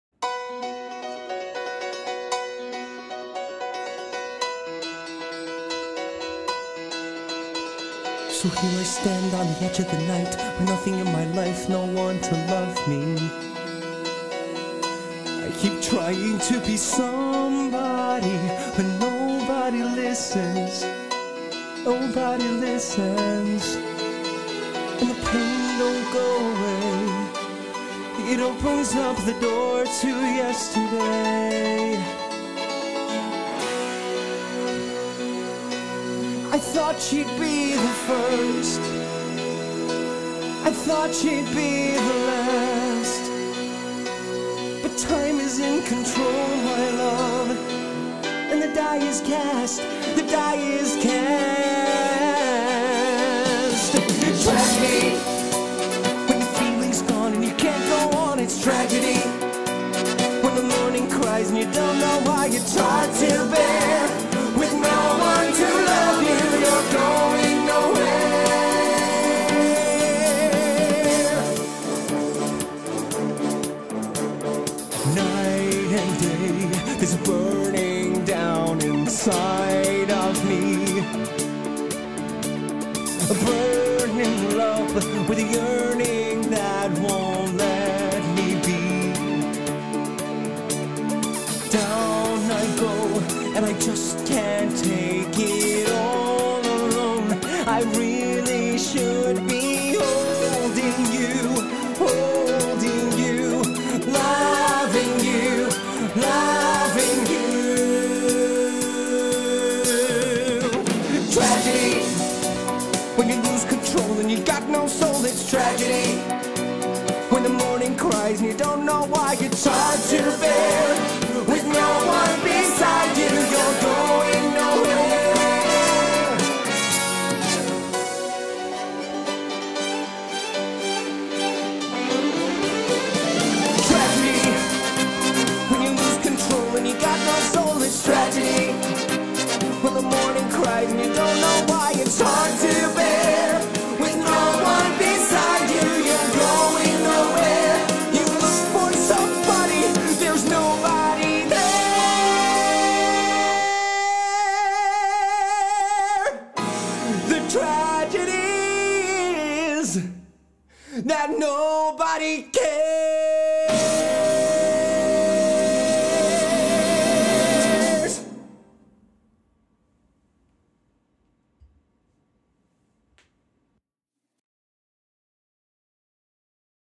Nick Hayes - Saturday Night Fever London Bobby C
Nick Hayes was 1st cover Bobby C in the 2005/6 West End cast of Saturday Night fever at the Apollo victoria theatre London